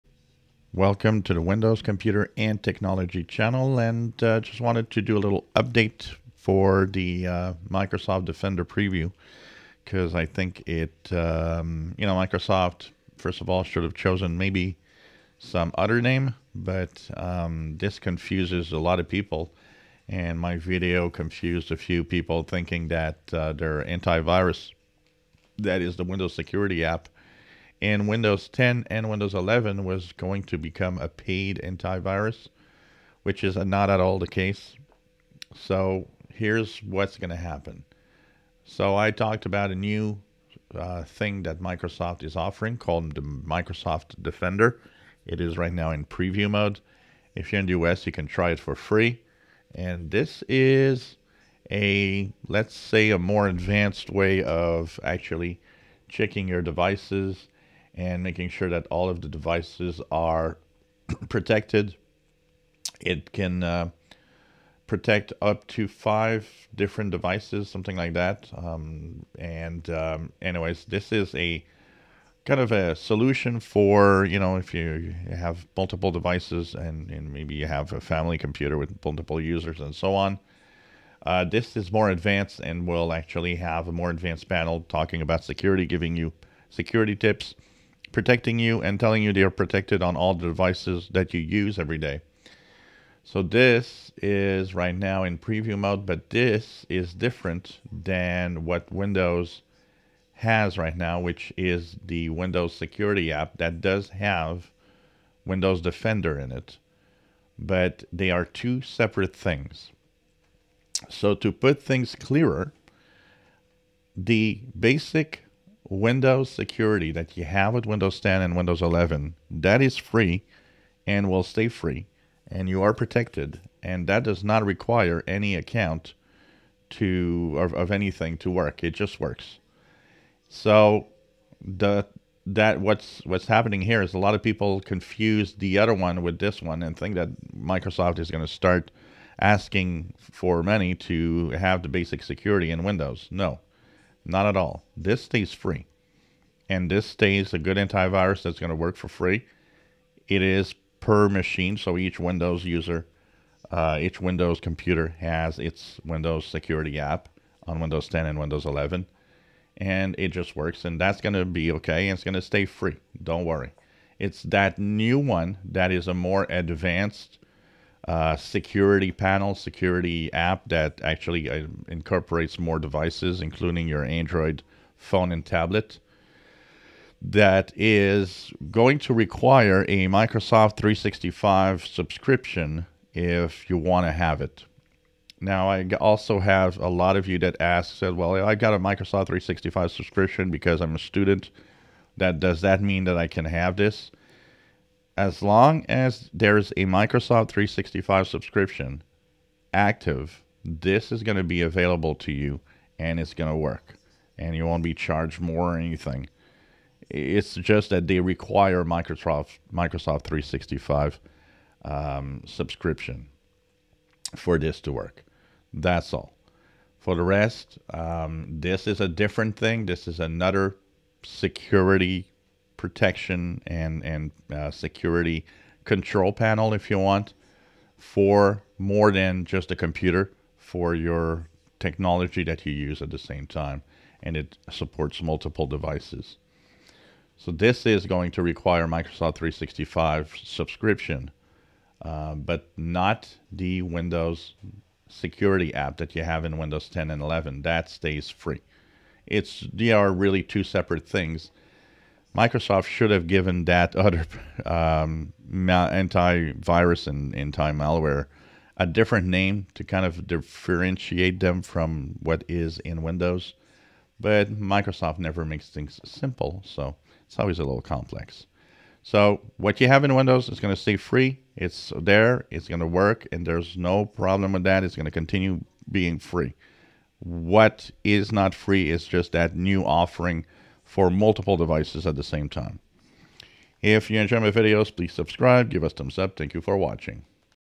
Microsoft Defender preview app VS Windows security app viewer questions answers
Windows Security that comes with Windows 10 and 11 is going to stay free.   Only the new Microsoft Defender app for multiple devices requires a Microsoft 365 subscription